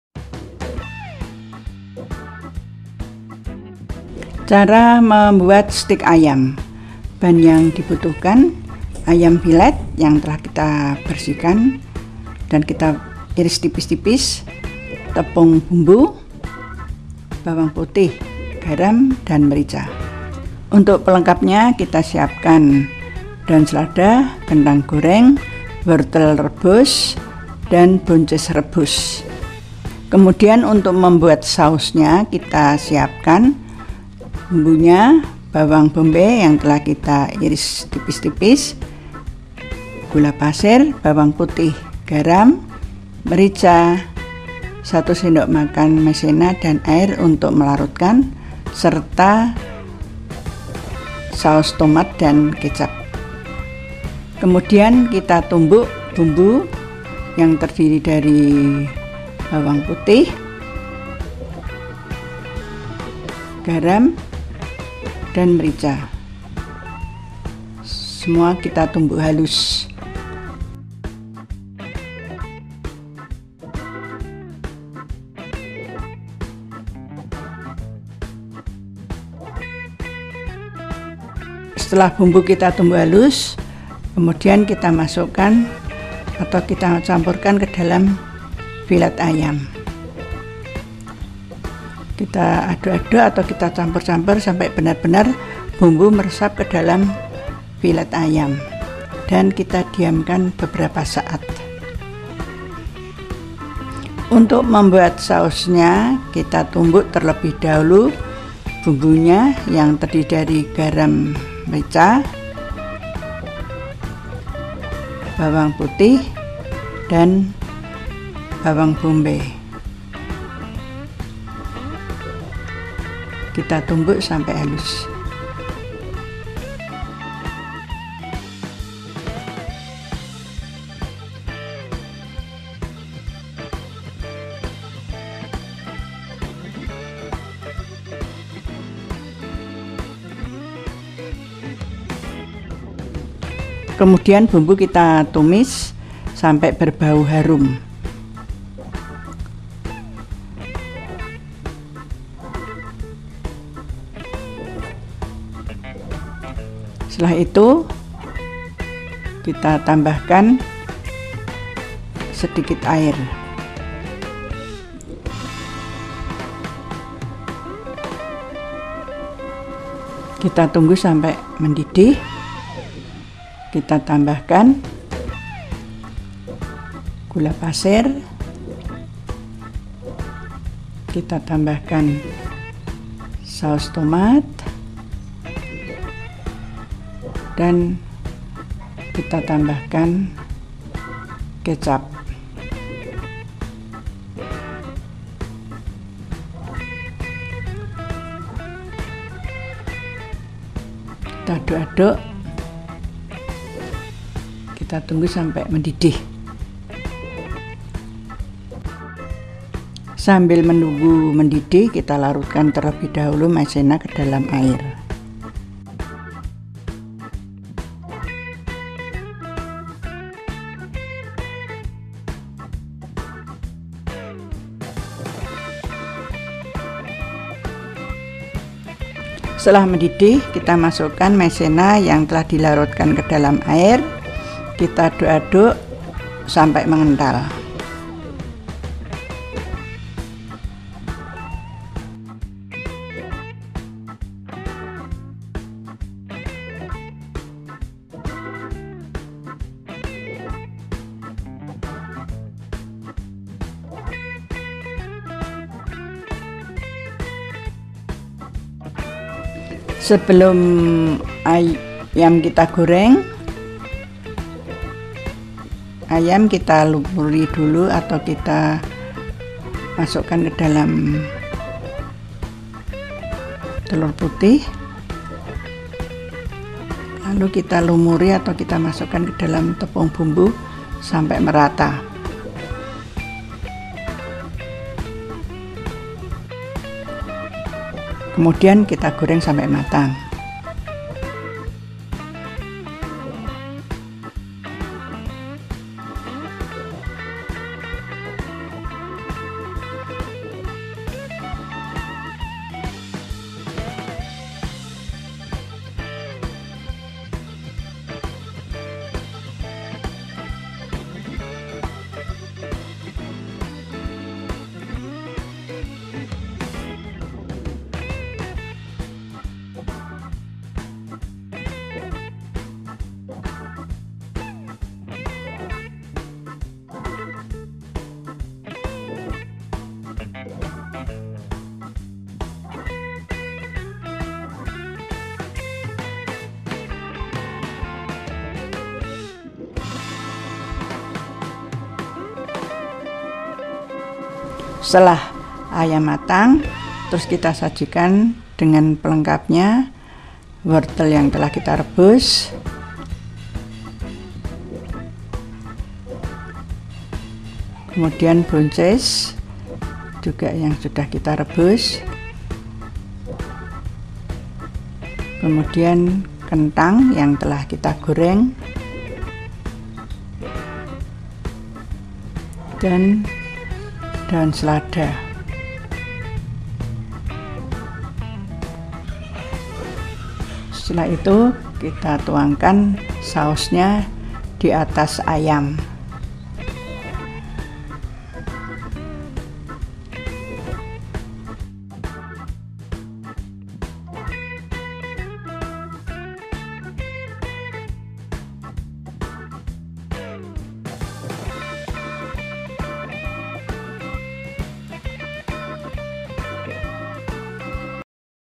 Cara Membuat & Resep Chicken Steak(Steak Ayam) Bahan Sederhana
ingin membuat steak ayam rumahan dengan bahan yang sederhana tapi tetap enak dan menggugah selera, silahkan lihat video ini